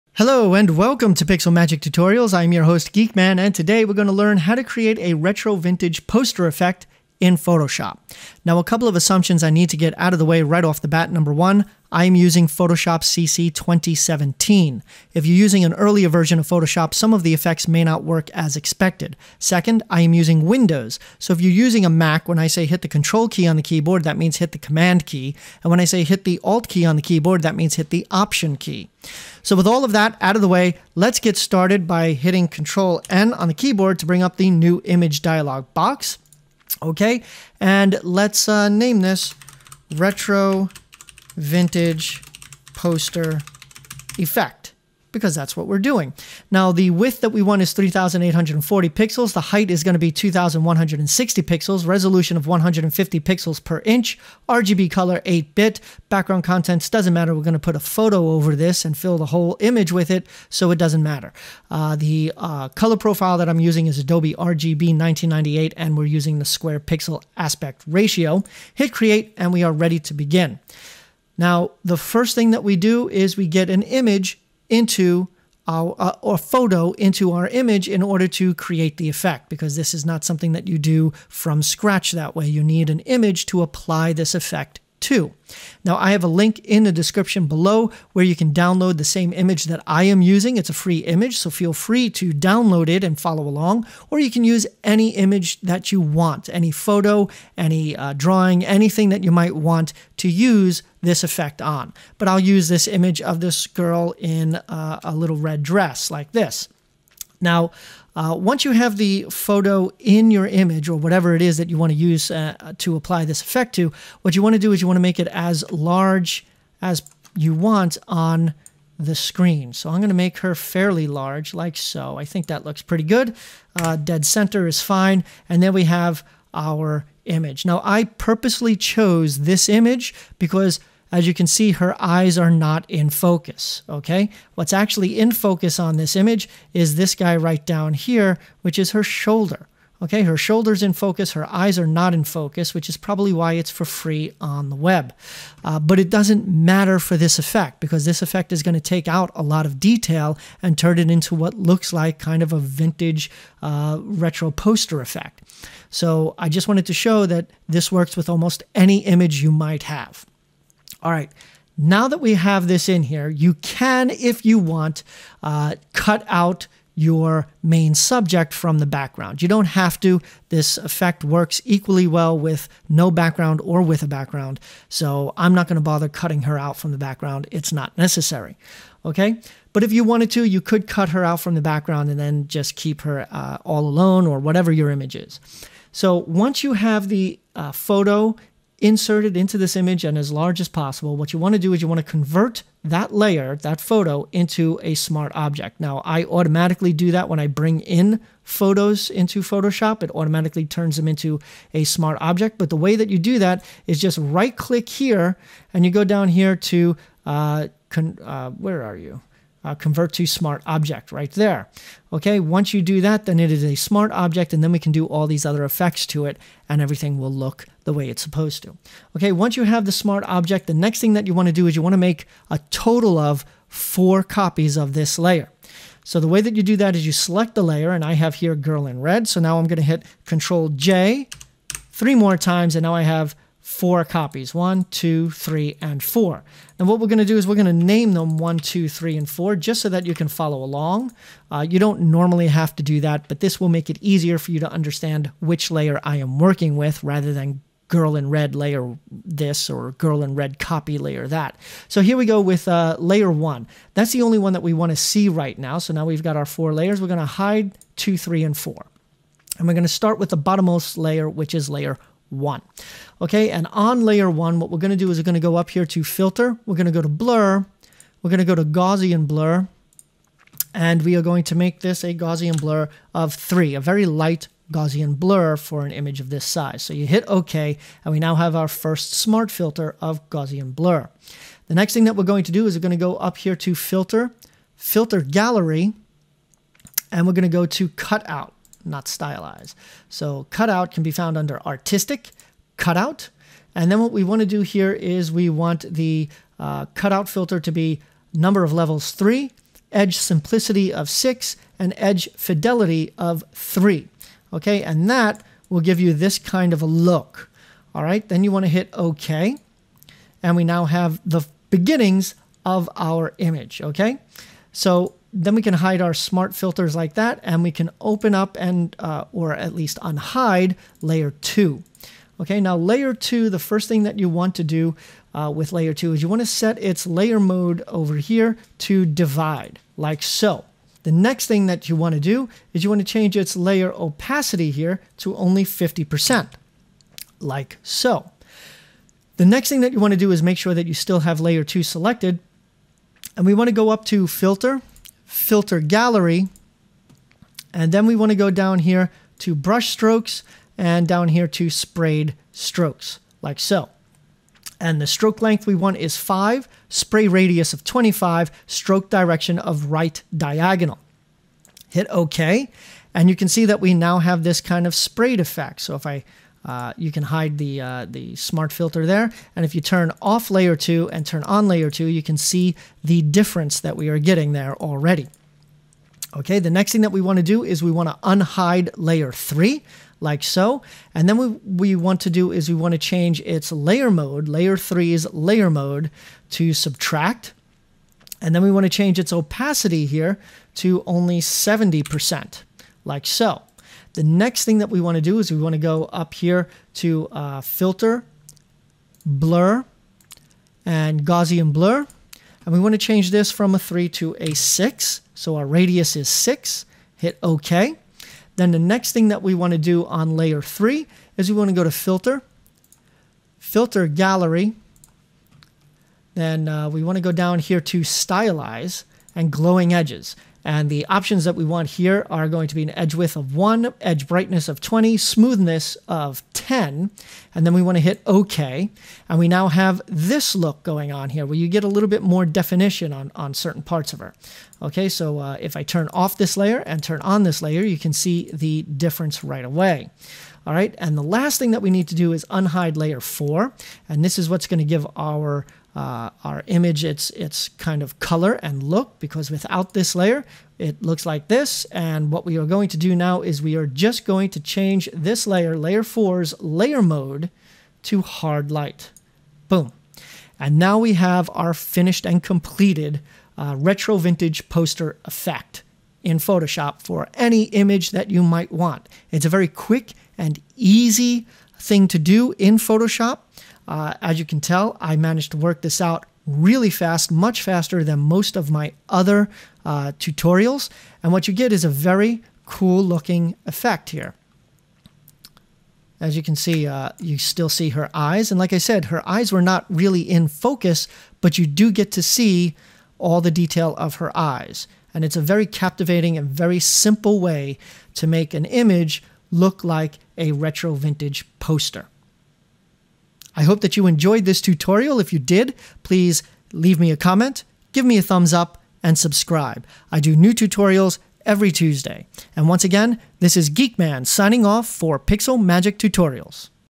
Retro Vintage Poster Effect - Photoshop Tutorial
In this Photoshop CC 2017 tutorial we'll learn how to create a fast and easy retro vintage poster effect.

►RESOURCE LINKS◄